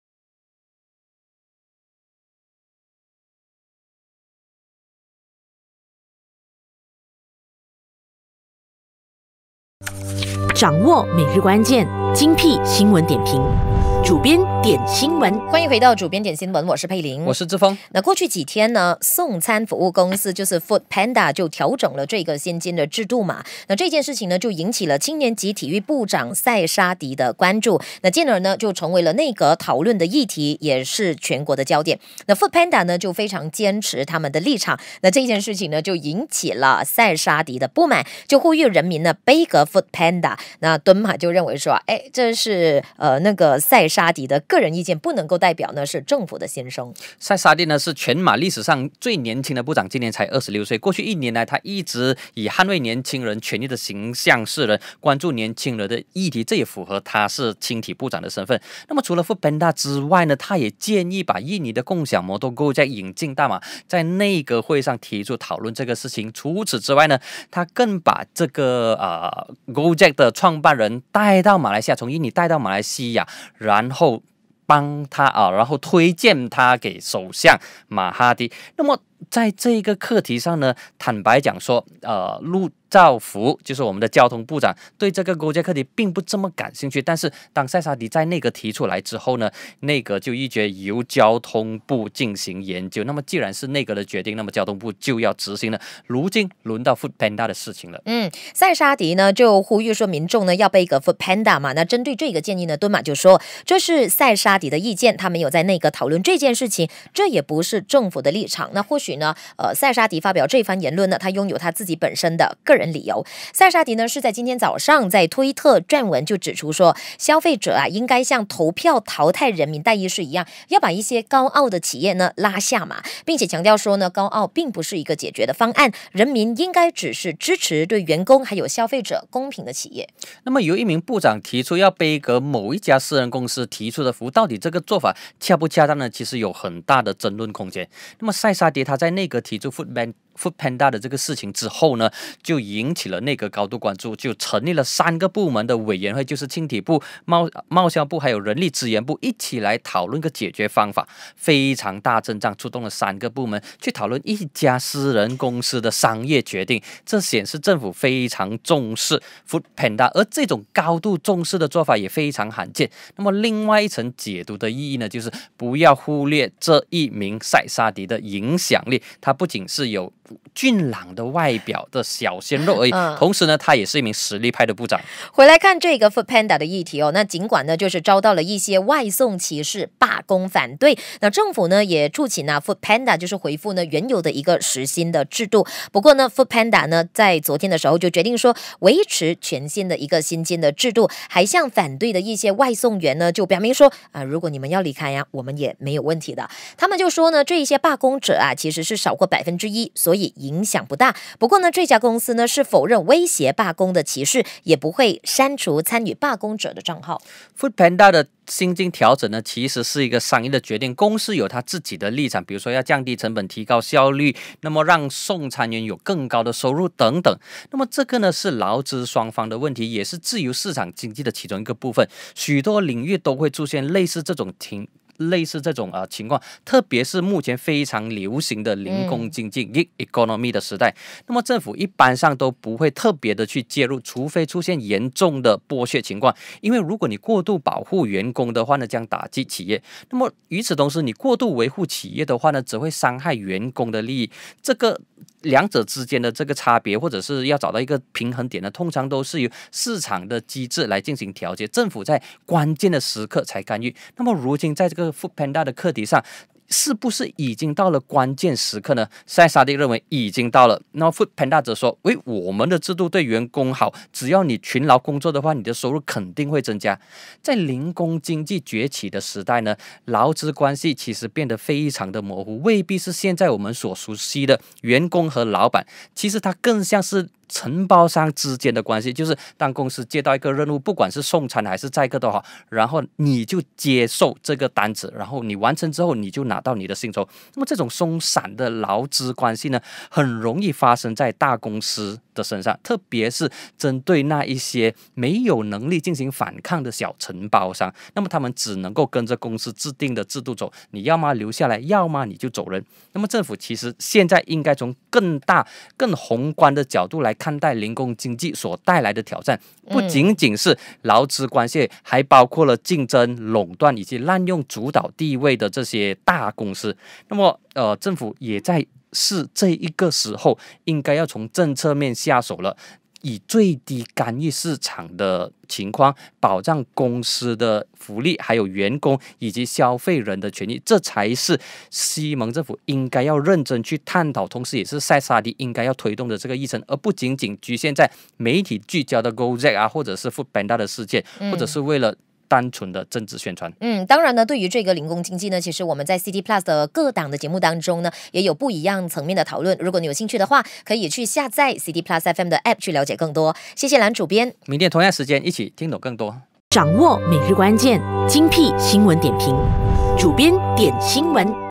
【主编点新闻 2019/10/8】 赛沙迪促杯葛Foodpanda 敦马：非政府立场
首相马哈迪认为，赛沙迪呼吁民众杯葛Foodpanda的立场是个人意见，不代表政府心声，内阁也没有讨论这课题。主编认为，政府应从宏观经济角度，以及政策层面下手，拟定零工经济领域的规范，在最低干预市场的情况下，保障公司、员工，以及消费者三方的权益。这是赛沙迪可以推动的议程！不应该局限在，吸引媒体焦点的Gojek和Foodpanda事件，又或者为了政治宣传。